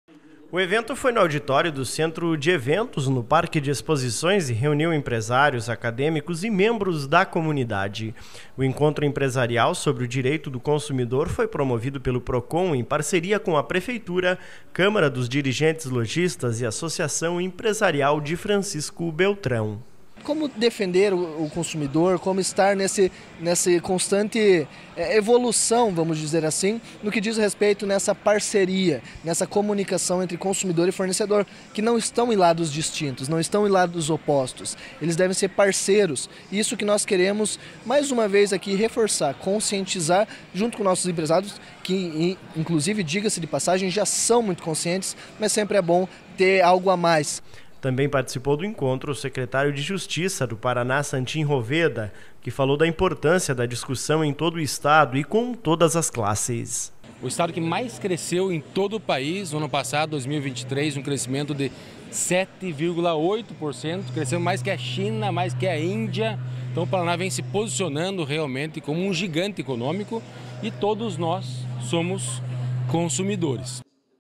Secretário de Justiça do Paraná participa de encontro na Expobel
Eventos: Santin Roveda participou do encontro empresarial sobre direito do consumidor promovido pelo Procon.